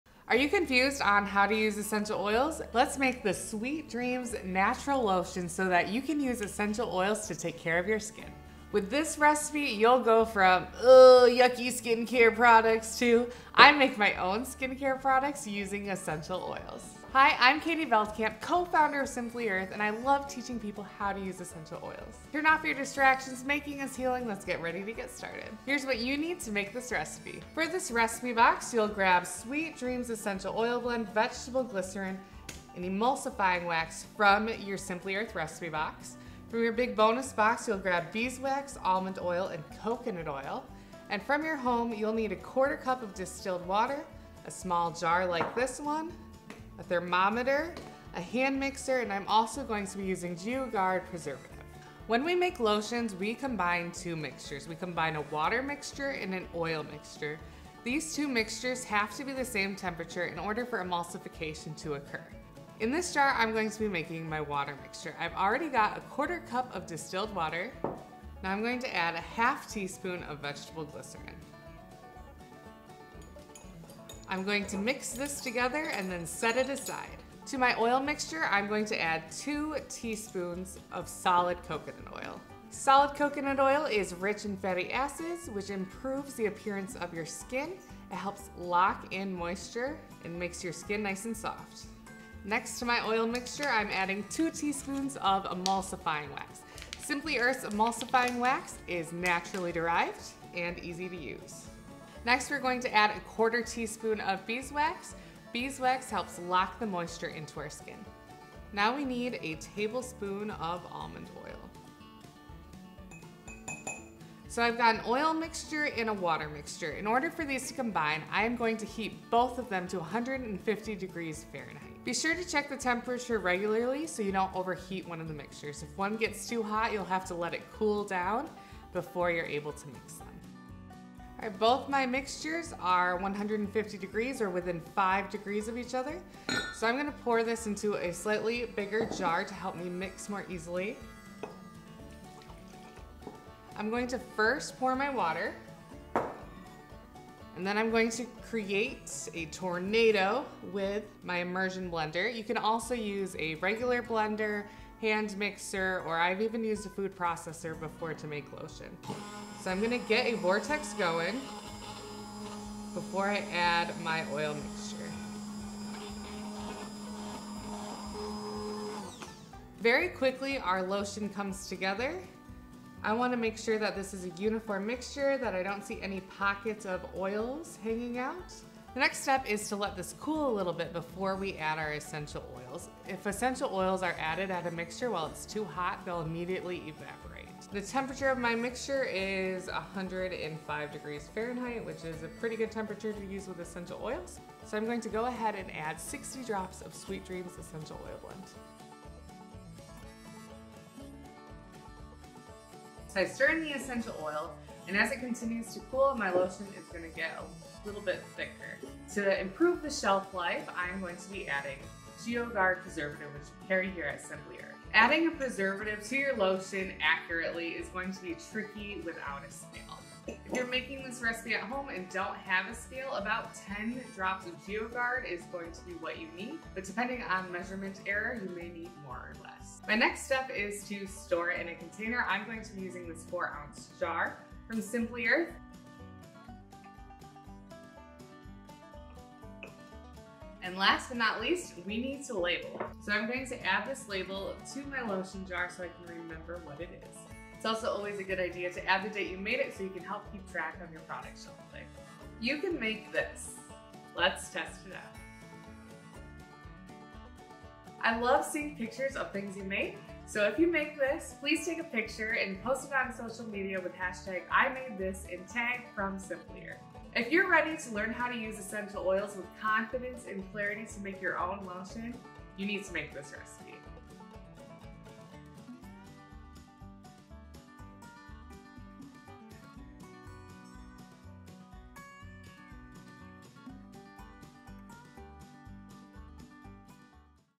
Sweet Dreams Natural Lotion
This Sweet Dreams Natural Lotion Recipe is what dreams are made of. If you're looking for a spa-quality lotion that you can make yourself, you are going to love this recipe. Made with essential oils and other nourishing ingredients, it's the perfect way to pamper your skin. And best of all, it's completely natural - no synthetic fragrances or colors! So if you're ready to give your skin some TLC, gather up these ingredients and get ready for sweet dreams.

Here's what you'll learn in this video:
■ How to use this recipe